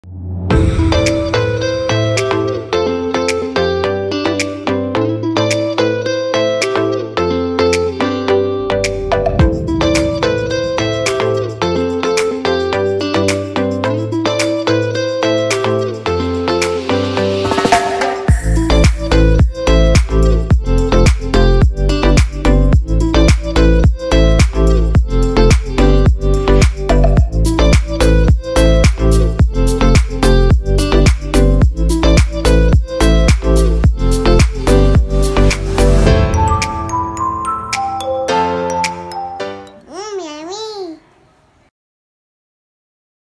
Garlic Mayo Dip/Sauce|Recipe by Pure Passion
Quick & easy recipe for garlic mayo sauce. You can enjoy it with fries, shawarma , bbq chicken, nuggets etc or in any kind of fast food.
Ingredients:
Mayonnaise
Salt
Vinegar
Black pepper
Garlic powder
Milk